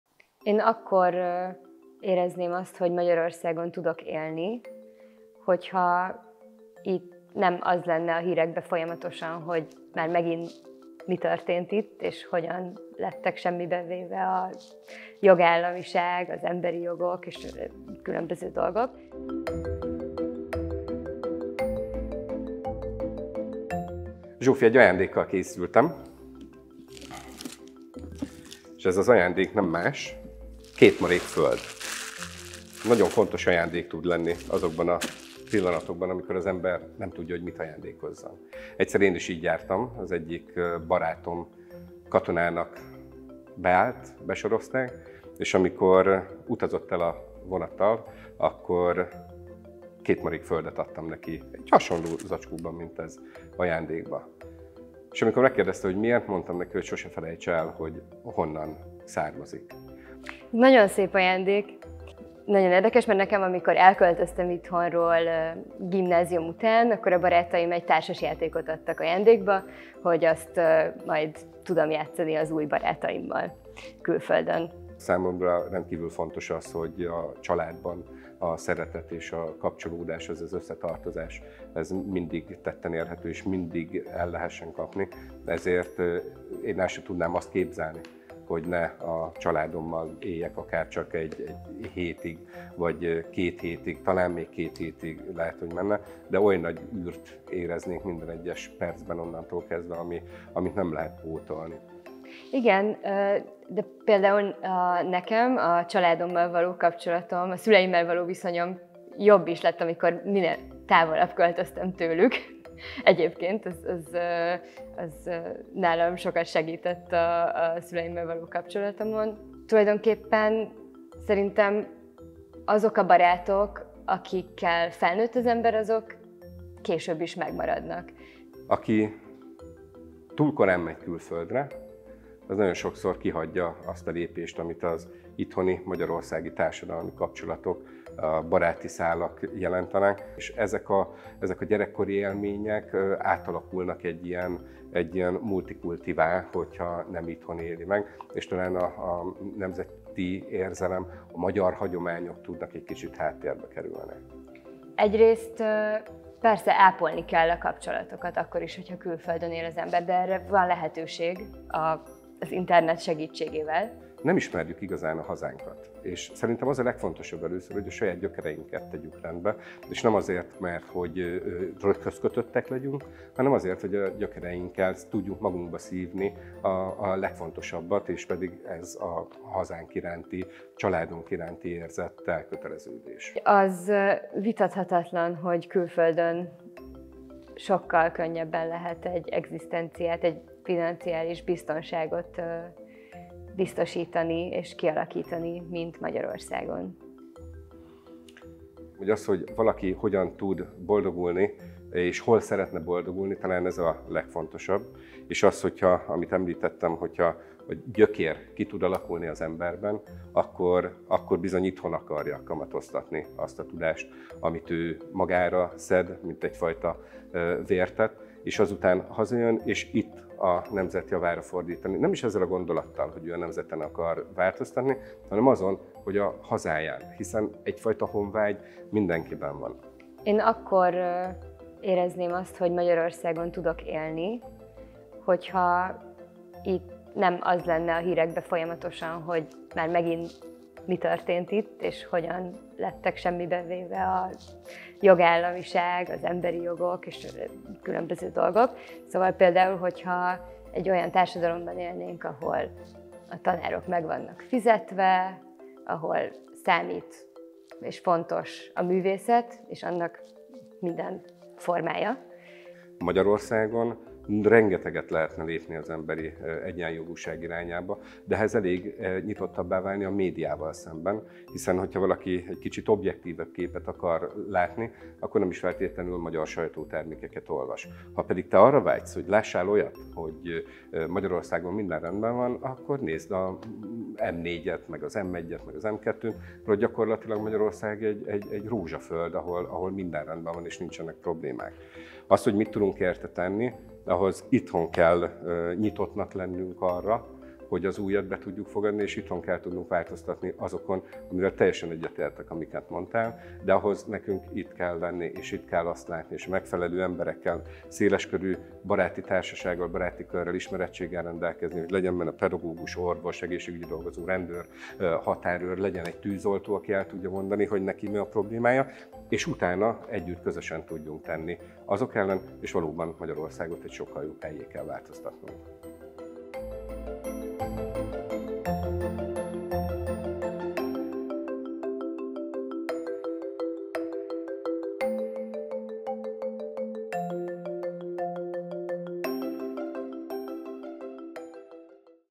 Elköltöznél külföldre a jobb élet reményében?
Sokfélék vagyunk, de szót értünk egymással. Elköltöznél külföldre azért, mert ott könnyebb a megélhetés és tisztelik az oktatást, a kultúrát? Inkább itthon maradnál, hogy változtass a rossz dolgokon és így segíts a családodon, a hazádon? Egy külföldre költözött, fiatal magyar lányt és egy itthon élő, családos férfit ültettünk egy stúdióba. Mindketten szeretik a hazájukat, de máshogyan. Te kinek adsz igazat?